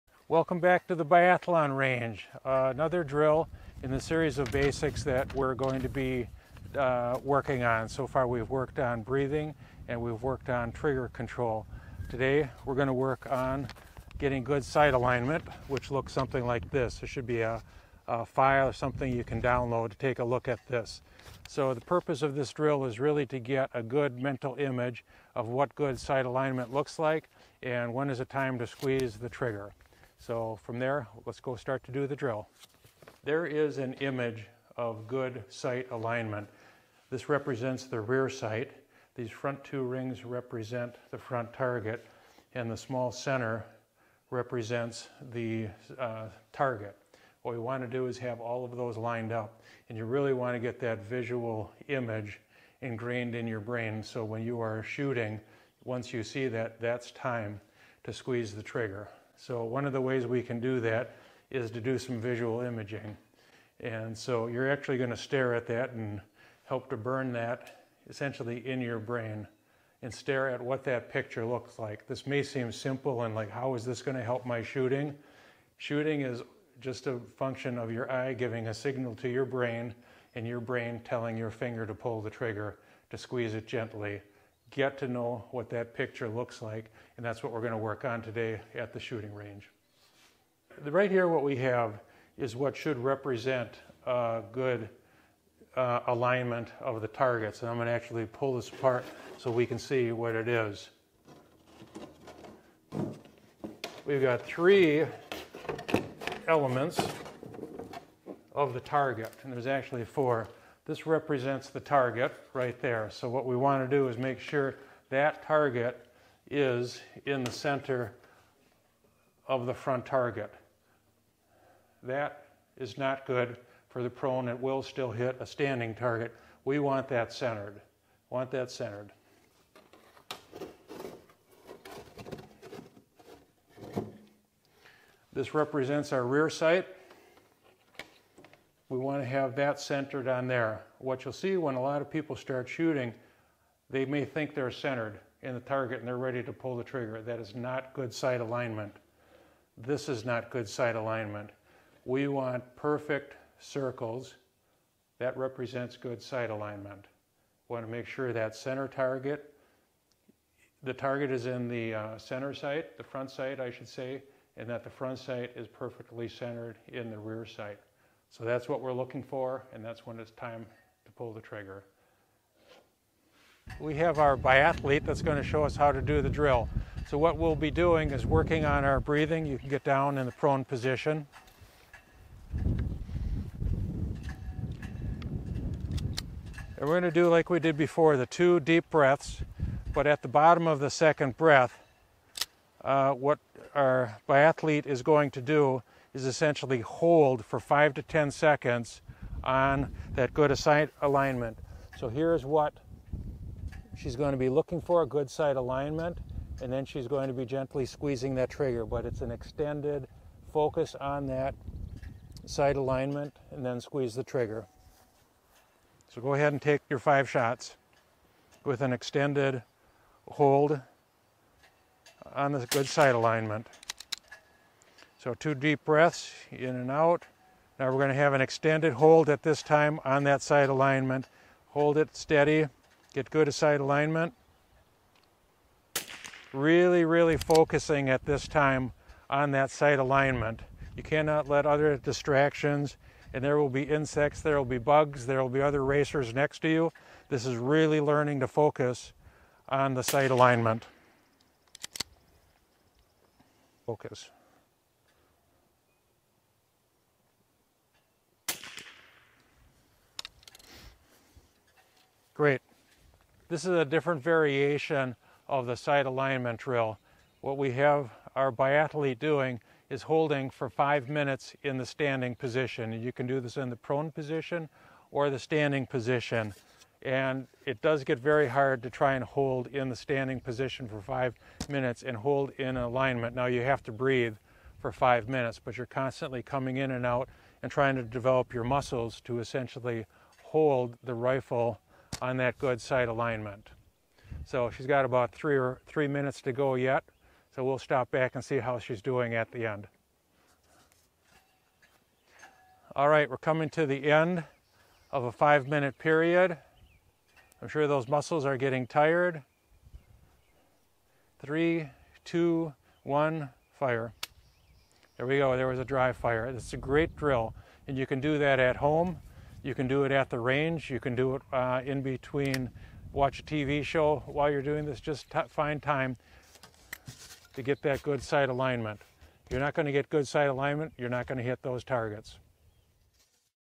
Baseline Shooting Drills: Sight Alignment
This is such a simple but critical drill and be done with or without a gun. It can be done during dry firing or with ammunition. The purpose of this drill is to develop good mental picture of good sight alignment and when to squeeze the trigger.

Good sight alignment consists of a series of concentric circles with the target in the center of the perfect circles. The attached image shows the elements of a good sight alignment. Look at it often and develop a good mental image of the picture. You will note that the front sight is perfectly centered in the rear sight and the target is centered in the front sight. Simple, right? Practice getting perfect sight alignment with a resting heart rate and then with an elevated heat rates up to race level. You will notice it gets harder and harder to keep good sight alignment the higher your heart rate is. Practice this in the prone position and the standing position. Prior to firing, focus on maintaining a good sight picture while maintain breathing and trigger control. It will be much harder standing. Train your brain to know subconsciously what good sight alignment is. Focus on the attached figure often, daily if you can.

When shooting work on breath control, trigger control and good sight alignment.

BASELINE SHOOTING SKILLS
The skills we are learning are essential to developing good shooting technique, the last thing in a biathlon race you want to think about is every detail of how to shoot. Practicing these drills so you can do them without thinking is critical to having fun in a biathlon race.